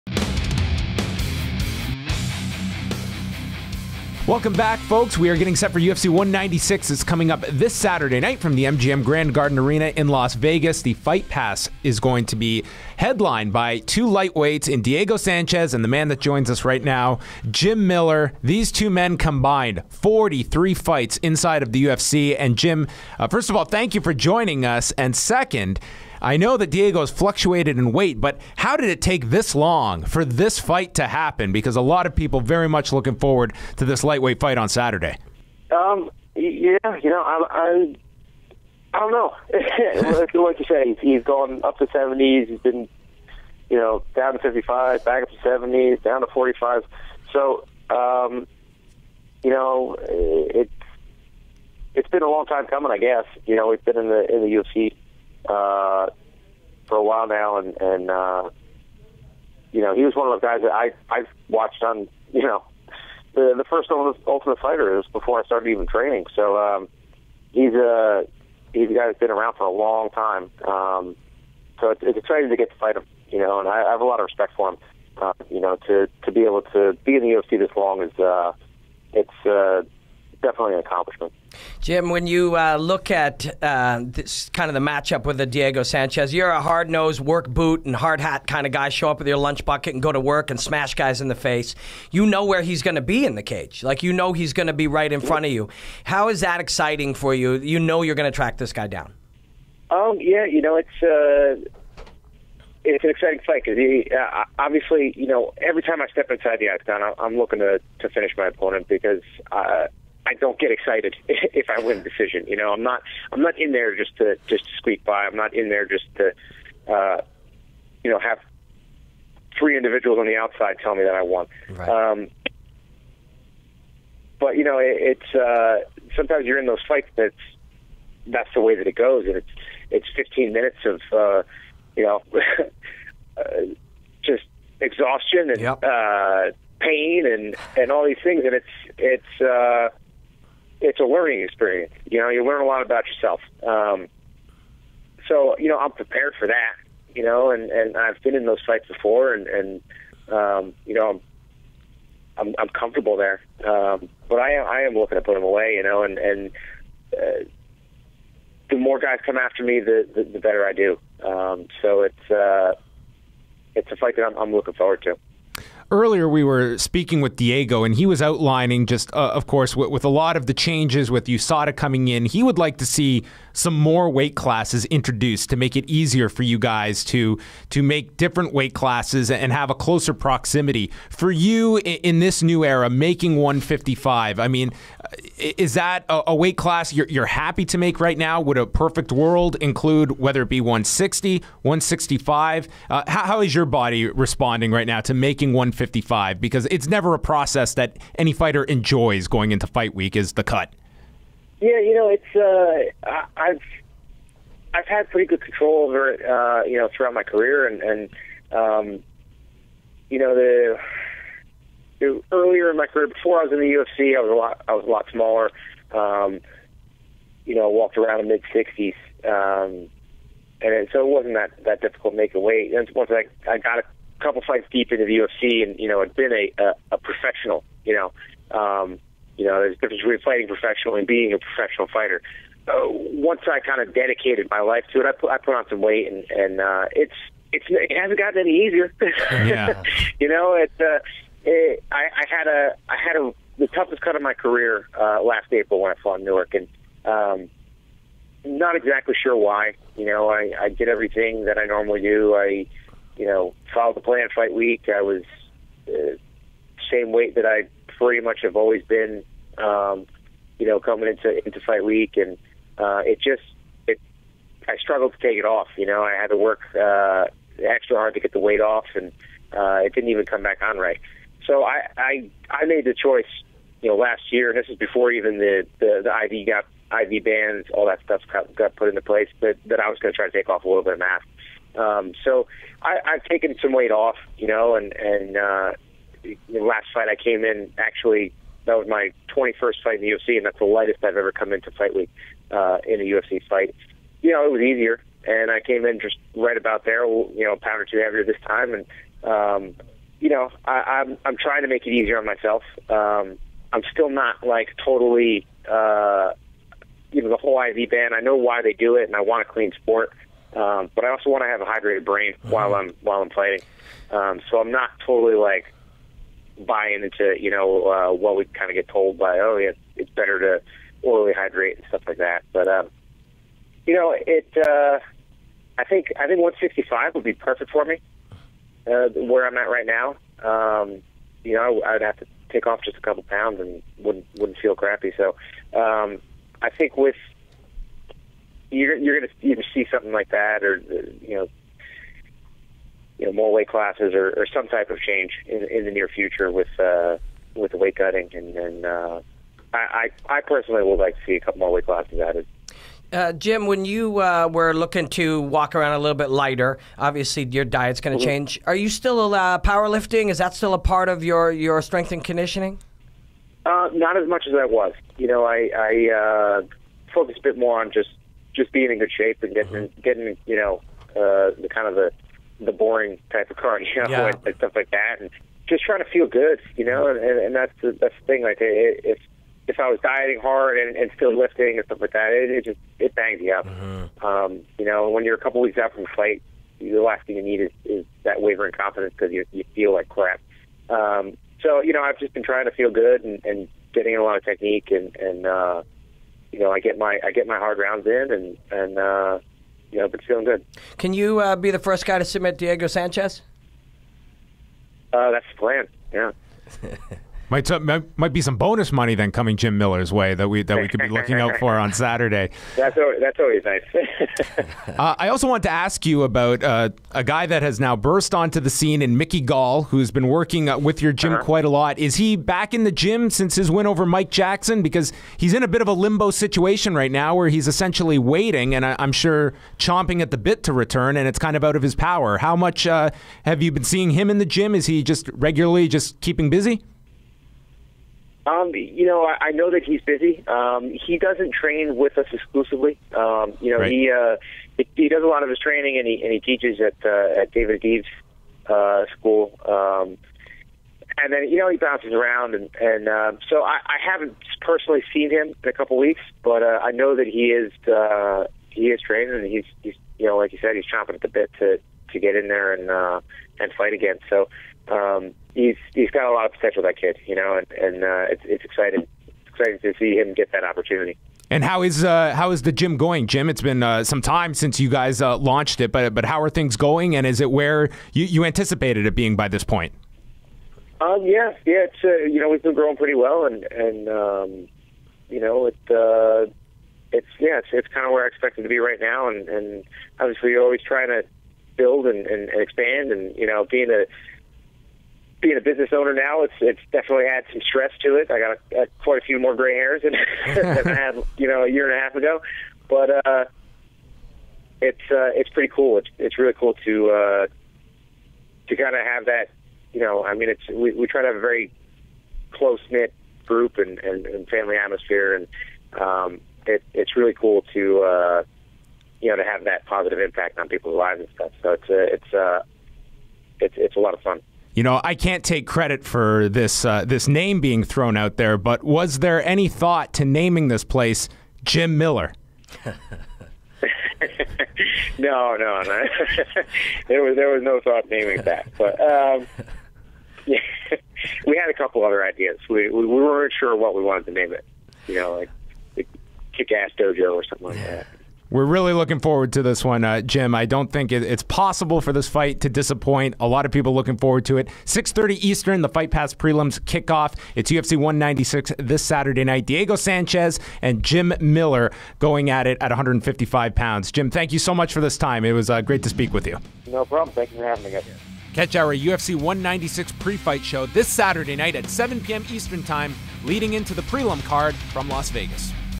UFC 196: Jim Miller - "Being in the UFC This Long is Definitely an Accomplishment"
- Jim Miller joins John Pollock and Robin Black to preview his bout against TUF 1 winner Diego Sanchez at UFC 196 discussing weight fluctuations for Sanchez, what to expect in the fight, training Mickey Gall and more.